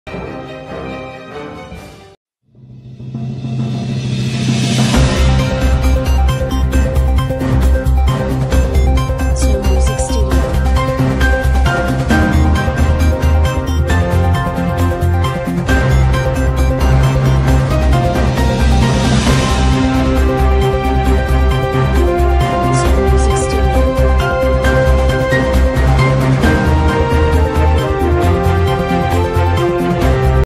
थोरासी सीकर का लाल राजेश भास्कर रिटायर से 5 दिन पहले हो गया शहीद | 31 जनवरी को हो रहे थे रिटायर
सीकर का एक और लाल शहीद

थोरासी सीकर का लाल राजेश भास्कर शहीद

5 दिन बाद होने वाला था रिटायर